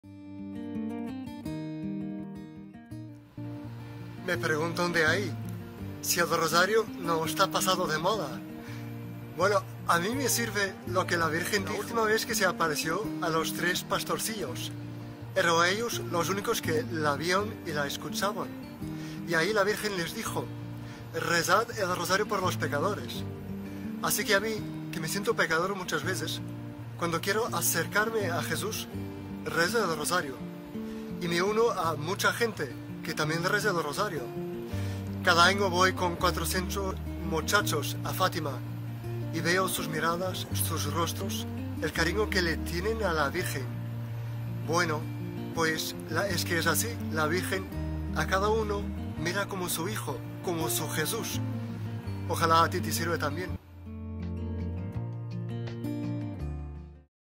La Virgen nos mira como sus hijos
Rezar el Rosario por los pecadores, también para acercarnos más a Jesús y unirnos por medio de este a mucha gente.